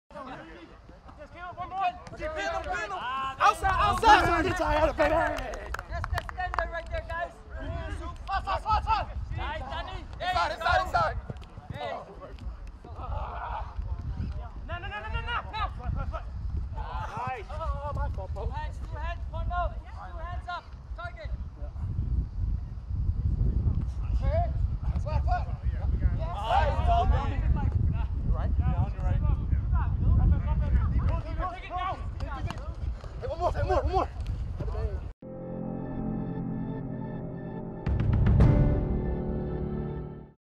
L-Drill Passing Grid: RugbyTown CROSSOVER Academy Glendale CO 8.9.21
L DRILL PASSING GRID